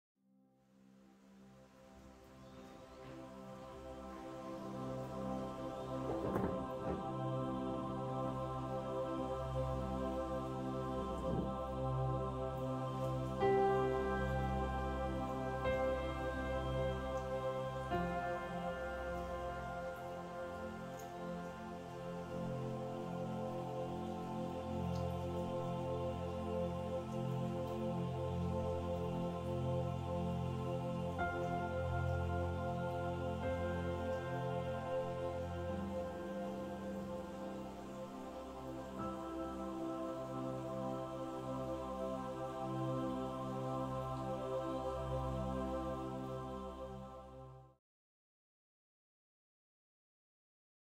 Desktop ZERO Preserved Moss Wall Terrarium by TerraLiving
Official Etsy store

How to display & interact with TerraLiving. Happy Winter Solstice 2020!